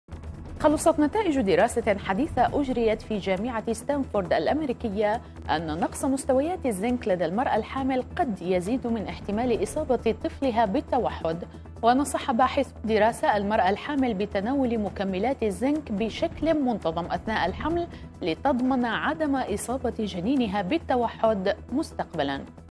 دراسة: نقص الزنك أثناء الحمل قد يؤدي للإصابة بالتوحد
خلصت نتائج دراسة حديثة أجريت في جامعة ستانفورد الأمريكية، بأن نقص مستويات الزنك لدى المرأة الحامل قد يزيد من احتمال إصابة طفلها بالتوحد، لذا ينصح العلماء المرأة الحامل بتناول مكملات الزنك بشكل منتظم أثناء الحمل لتضمن عدم إصابة جنينها بالتوحد مستقبلاً.

وذكرت الدكتورة سالي كيم من كلية الطب بجامعة ستانفورد في كاليفورنيا "يرتبط التوحد بأنواع معينة من الجينات التي تشارك في تكوين ونضج وتثبيت نقاط التشابك العصبي أثناء المراحل الأولى لتطور الجنين".

وأضافت "تربط النتائج التي توصلنا إليها، مستويات الزنك في الخلايا العصبية، بالإصابة بمرض التوحد".
من جهته قال البروفيسور كريغ غارنر، من المركز الألماني للأمراض العصبية "حتى وقتنا الحاضر لم يثبت قطعياً بأن نقص مستويات الزنك لدى الحامل، يؤدي حتماً لإصابة الجنين بالتوحد، ولا زلنا بحاجة لإجراء المزيد من الدراسات حتى نعتمد هذه النتائج بشكل نهائي".

يذكر بأن الزنك الذي يتواجد في اللحوم والمحار ومنتجات الألبان والخبز والحبوب، يساهم في صنع خلايا وأنزيمات جديدة، واستخلاص الكربوهيدات والدهون والبروتين من الطعام، إضافة إلى مساهمته في التئام الجروح.

وتقول هيئة الصحة الوطنية البريطانية، إن معظم الناس يحصلون على ما يكفي من الزنك من الأطعمة، ولا ينبغي أن يتناول المرء أكثر من 25 ملغ من مكملات الزنك يومياً.
كما وجدت الدراسة التي نشرت في دورية فرونتيرز المختصة بعلم الأعصاب الجزيئي، بأن الزنك يدخل الخلايا العصبية المستهدفة عبر نقطة التشابك العصبية، ويربط نوعين هامين من البروتينات التي تُحدث تغيرات في التكوين والوظيفة للخلايا العصبية في مرحلة التطور، وفق ما نقلت صحيفة ديلي ميل البريطانية.